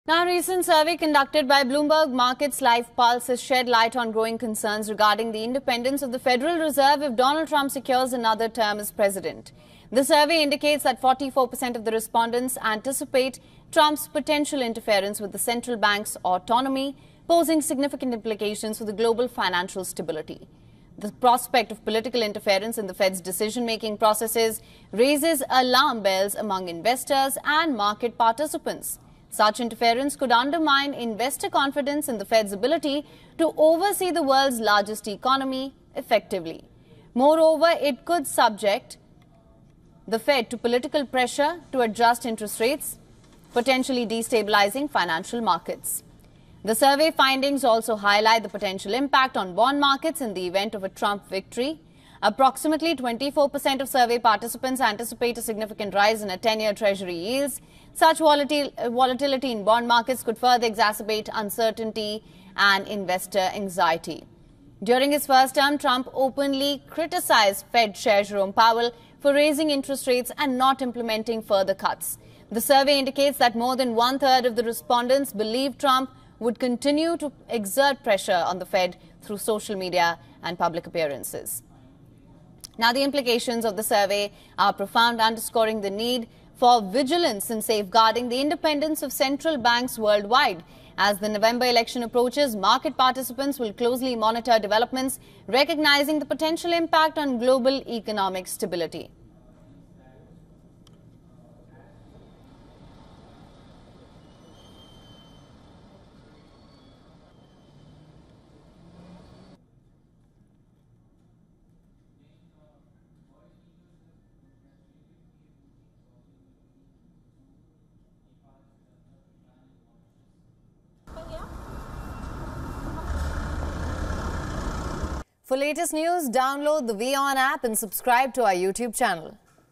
Trump's reelection threatens FED independence? | World Business Watch | WION
A recent survey conducted by Bloomberg Markets Live Pulse has shed light on growing concerns regarding the independence of the Federal Reserve if Donald Trump secures another term as President. The survey indicates that 44% of respondents anticipate Trump's potential interference with the central bank's autonomy, posing significant implications for global financial stability. The prospect of political interference in the fed's decision-making processes raises alarm bells among investors and market participants. Such interference could undermine investor confidence in the Fed's ability to oversee the world's largest economy effectively. Moreover, it could subject the Fed to political pressure to adjust interest rates, potentially destabilizing financial markets.